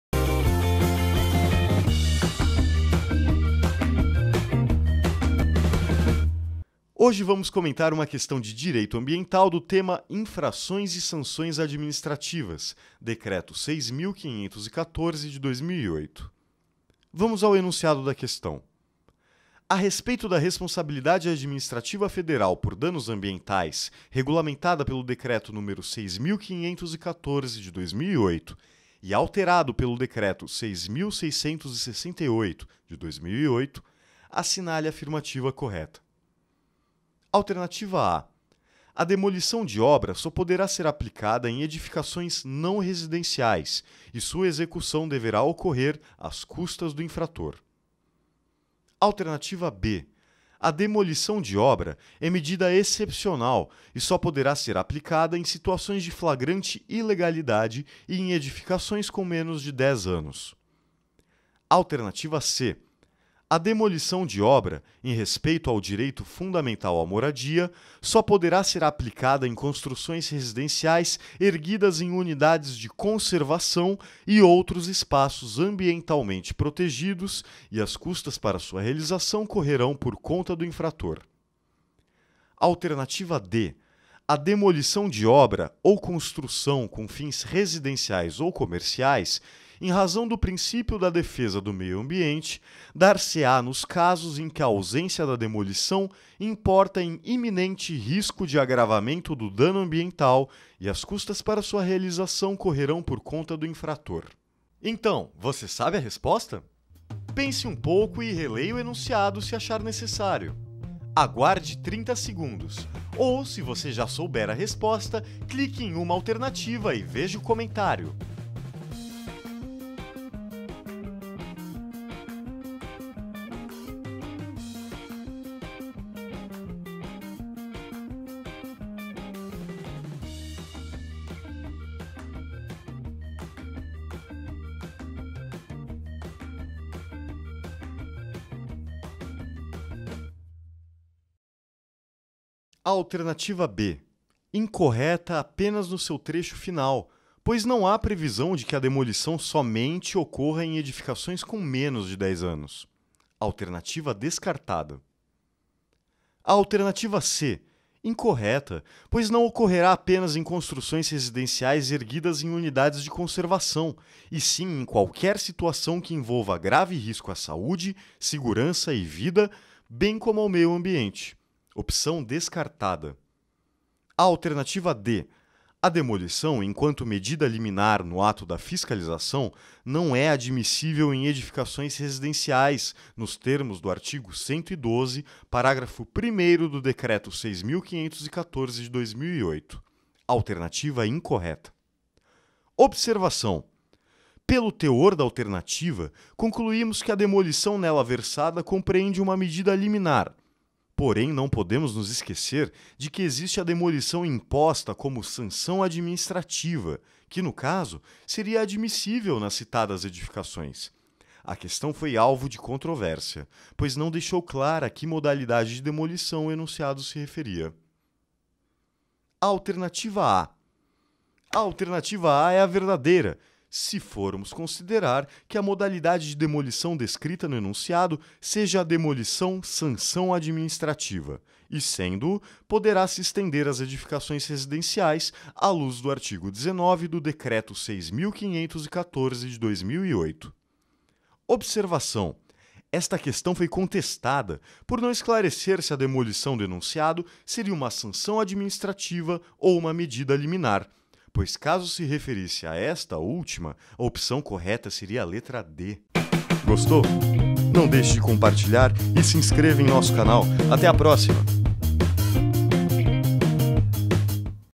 Questão Comentada - FGV - Direito Ambiental - Decreto 6.514/08 - DAM 2 D6 001
A respeito da responsabilidade administrativa federal por danos ambientais, regulamentada pelo Decreto n. 6.514/08 e alterado pelo Decreto 6.686/08, assinale a afirmativa correta.

A) A demolição de obra só poderá ser aplicada em edificações não residenciais e sua execução deverá ocorrer às custas do infrator.

B) A demolição de obra é medida excepcional e só poderá ser aplicada em situações de flagrante ilegalidade e em edificações com menos de dez anos.

C) A demolição de obra, em respeito ao direito fundamental à moradia, só poderá ser aplicada em construções residenciais erguidas em unidades de conservação e outros espaços ambientalmente protegidos e as custas para a sua realização correrão por conta do infrator.

D) A demolição de obra ou construção com fins residenciais ou comerciais, em razão do princípio da defesa do meio ambiente, dar-se-á nos casos em que a ausência da demolição importa em iminente risco de agravamento do dano ambiental e as custas para sua realização correrão por conta do infrator.